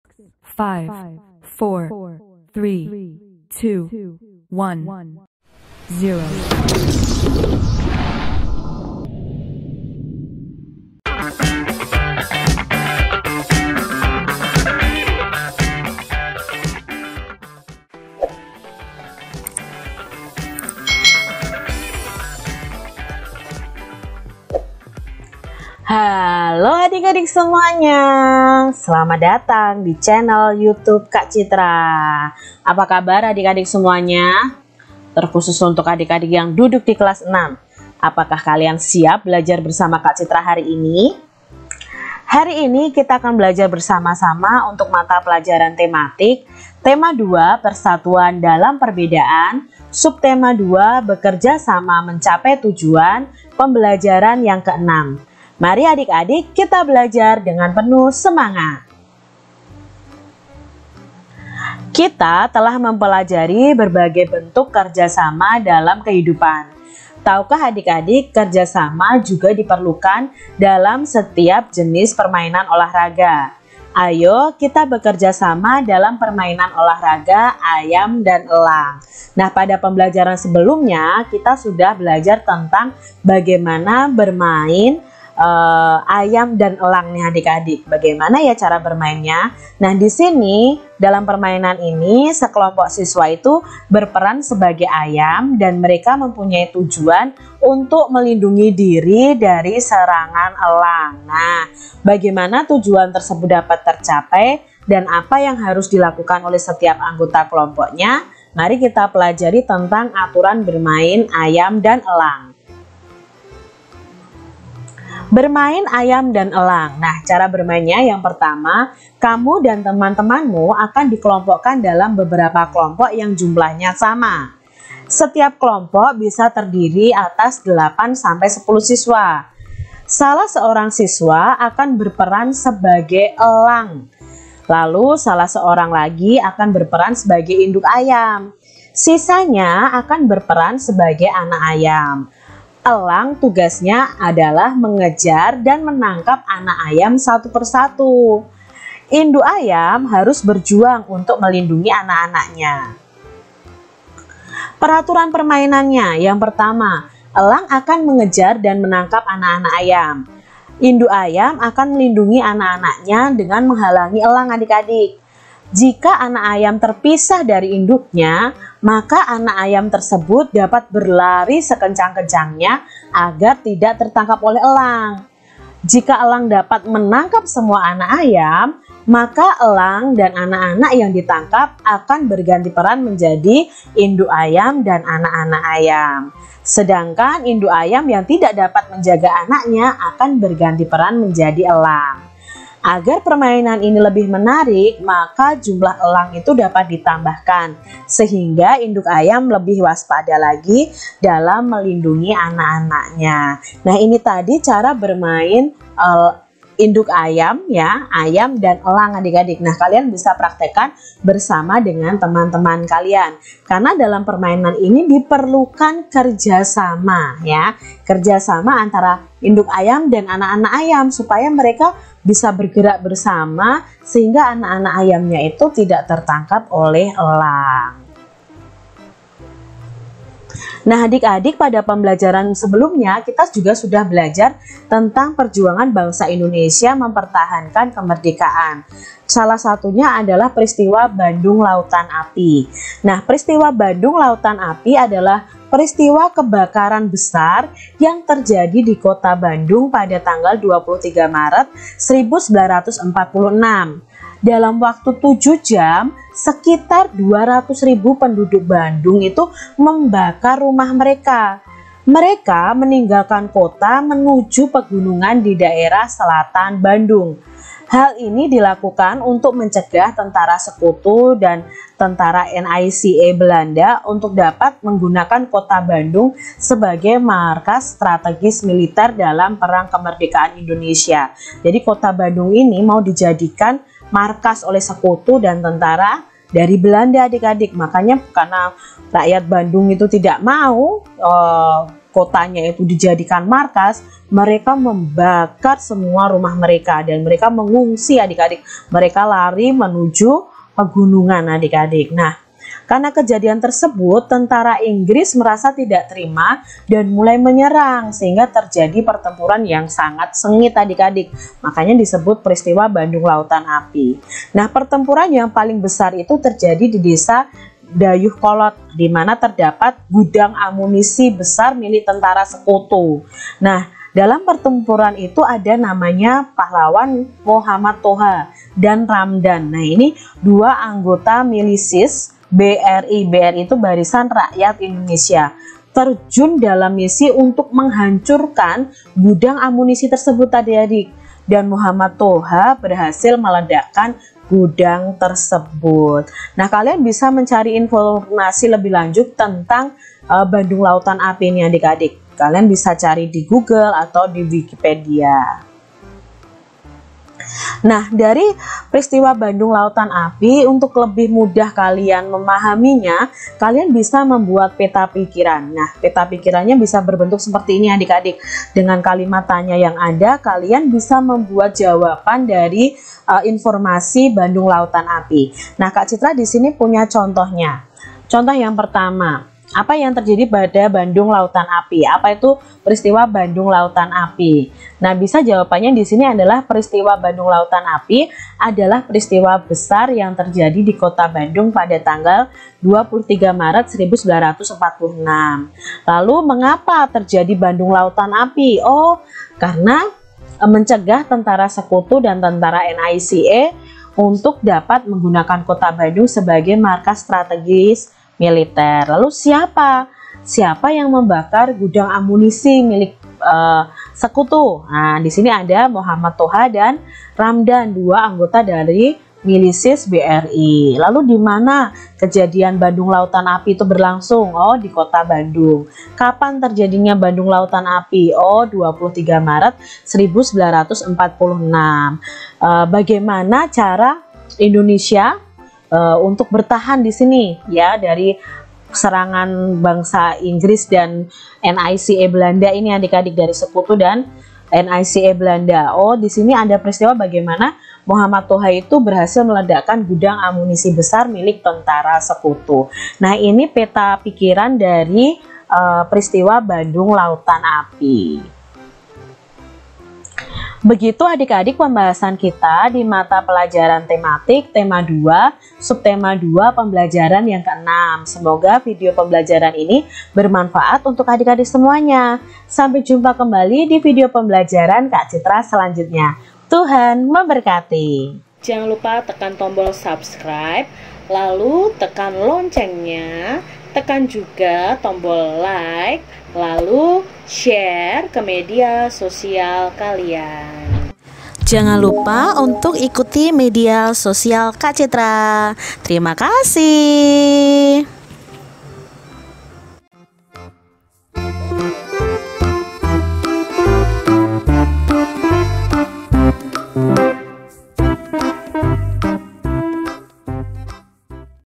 Kelas 6 Tematik : Tema 2 Subtema 2 Pembelajaran 6 (Persatuan Dalam Perbedaan)
Tema 2 Persatuan Dalam Perbedaan
Subtema 2 Bekerja Sama Mencapai Tujuan
Pembelajaran 6

Pembelajaran ini :
a) Sumber : Buku Tematik Pemerintah Kurikulum 2013 Tema 2 Kelas 6 Revisi 2018